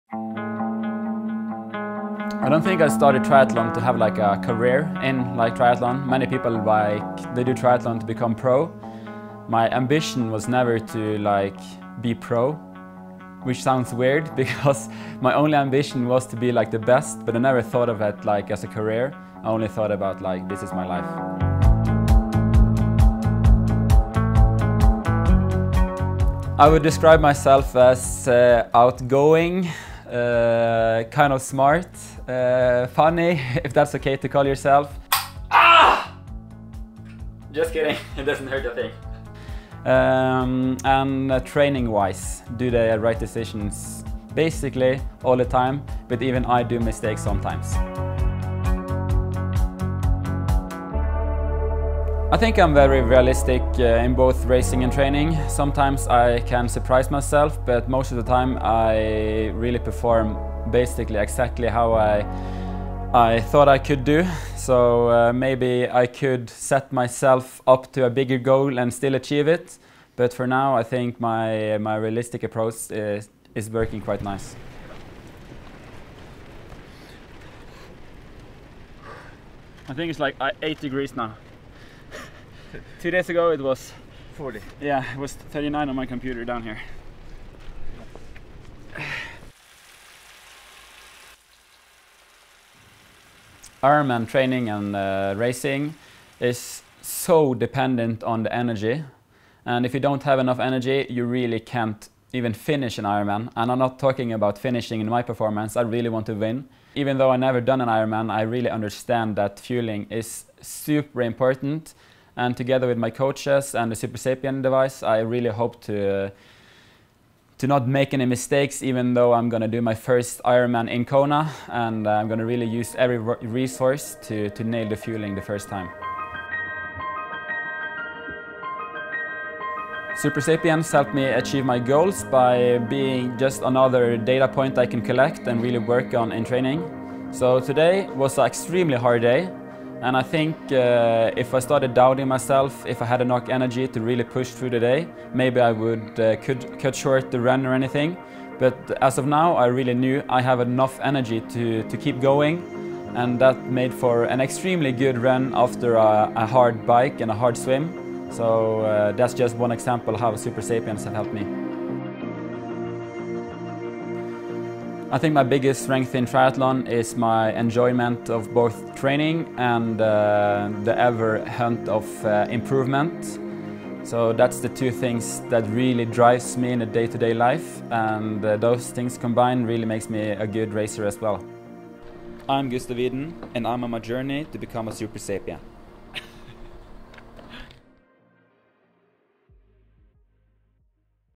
Eurovision to World Champion with Gustav Iden
Gustav has quickly made a name for himself on the international triathlon scene and in 2019 became the youngest Ironman 70.3 World Champion ever at just 23 years of age. The young Norwegian is now focused on the upcoming Olympic Games, and is using continuous glucose visibility with CGM to balance the performance demands of Ironman and the Olympics.

"I almost came to a complete stop to make sure I replenished, because I knew how important those carbs were. I’d love to see my glucose chart if I’d kept going versus me stopping to get my fueling back on track." Gustav understands the importance of his nutrition strategy -- Supersapiens gives him the visibility and peace of mind that his plan is working or not, so he can take on the world's biggest stages with endless energy.